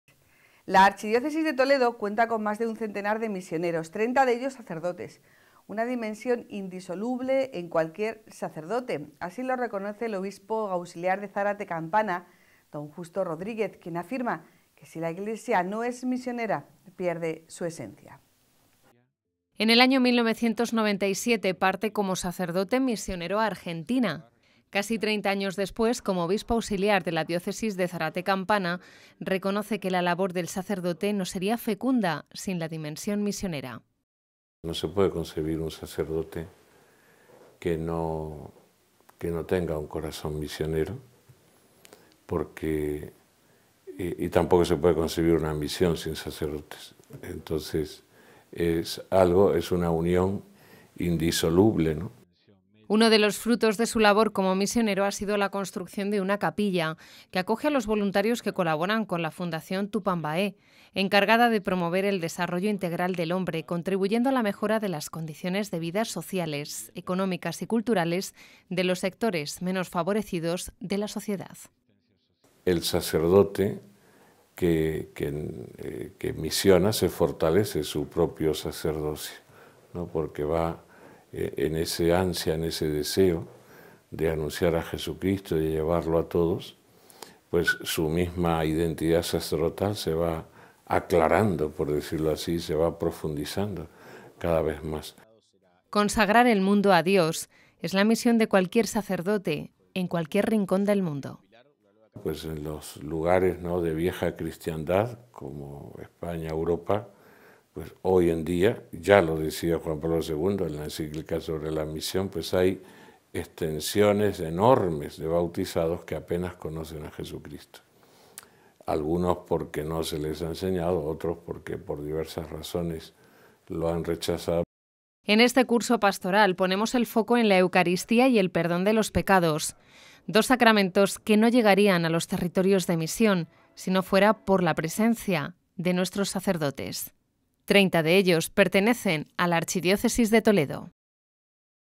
Ser sacerdote en la Misión
La dimensión misionera del sacerdote va indisolublemente unida a su identidad y carácter sacerdotal. Así lo reconoce el Obispo de Zarate-Campana, don Justo Rodríguez.